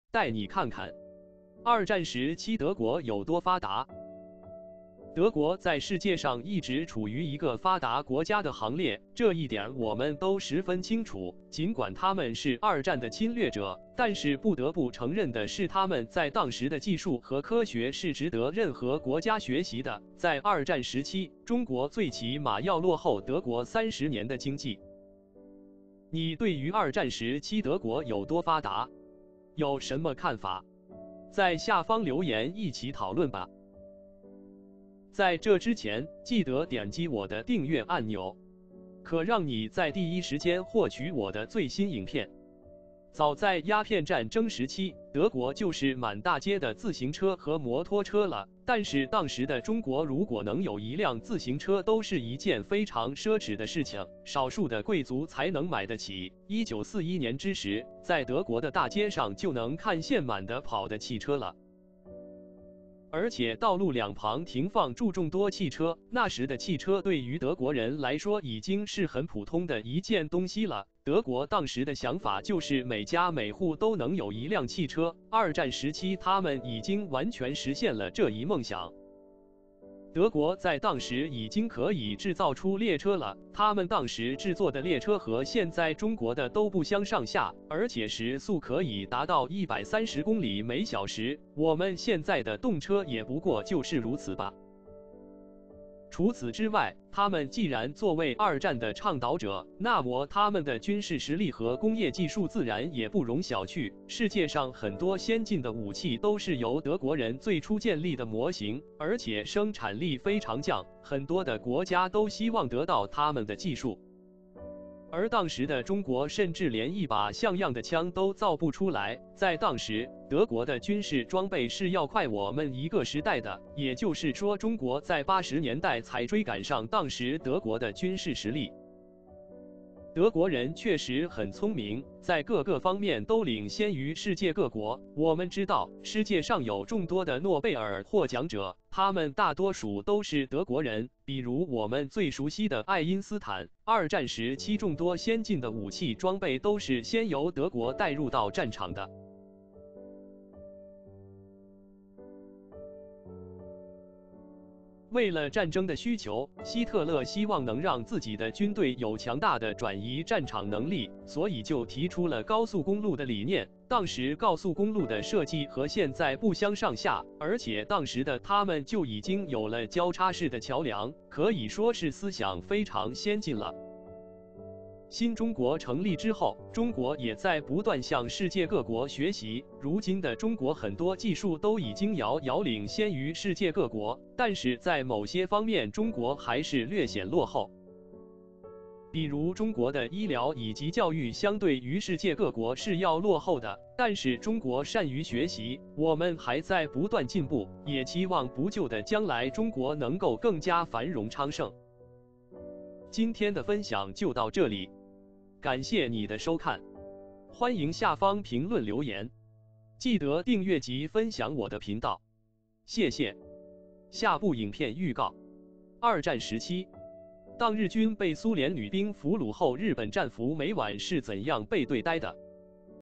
帶你看看！[二戰時期]【德國】有多發達？
帶你看看！二戰時期德國有多發達？

德國在世界上一直處於一個發達國家的行列，這一點我們都十分清楚，儘管他們是二戰的侵略者，但是不得不承認的是他們在當時的技術和科學是值得任何國家學習的，在二戰時期，中國最起碼要落後德國30年的經濟。

早在鴉片戰爭時期，德國就是滿大街的自行車和摩托車了，但是當時的中國如果能有一輛自行車都是一件非常奢侈的事情，少數的貴族才能買的起，1941年之時，在德國的大街上就能看見滿地跑的汽車了，而且道路兩旁停放著眾多汽車，那時的汽車對於德國人來說已經是很普通的一件東西了，德國當時的想法就是每家每戶都能有一輛汽車，二戰時期他們已經完全實現了這一「夢想」。

德國在當時已經可以製造出列車了，他們當時製作的列車和現在中國的都不相上下，而且時速可以達到130公里/每小時，我們現在的動車也不過就是如此吧。

除此之外，他們既然作為二戰的倡導者，那麼他們的軍事實力和工業技術自然也不容小覷，世界上很多先進的武器都是由德國人最初建立的模型，而且生產力非常強，很多的國家都希望得到他們的技術，而當時的中國甚至連一把像樣的槍都造不出來，在當時，德國的軍事裝備是要快我們一個時代的，也就是說中國在80年代才追趕上當時德國的軍事實力。

德國人確實很聰明，在各個方面都領先於世界各國，我們知道，世界上有眾多的諾貝爾獲獎者，他們大多數都是德國人，比如我們最熟悉的愛因斯坦，二戰時期眾多先進的武器裝備都是先由德國帶入到戰場的。

除此之外，德國在當時的交通已經非常發達，很多國家都是望塵莫及，而且當時的他們已經有高速公路了，而且高速公路這一理念也是由德國開創的，說到底這一點要歸功於希特勒，為了戰爭的需求，希特勒希望能讓自己的軍隊有強大的轉移戰場能力，所以就提出了高速公路的理念，當時告訴公路的設計和現在不相上下，而且當時的他們就已經有了交叉式的橋樑，可以說是思想非常先進了。

新中國成立之後，中國也在不斷向世界各國學習，如今的中國很多技術都已經遙遙領先於世界各國，但是在某些方面中國還是略顯落後，比如中國的醫療以及教育相對於世界各國是要落後的，但是中國善於學習，我們還在不斷進步，也期望不就的將來中國能夠更加繁榮昌盛

內容來源網絡并加以作品進行二次創作，如有侵權請聯繫刪除！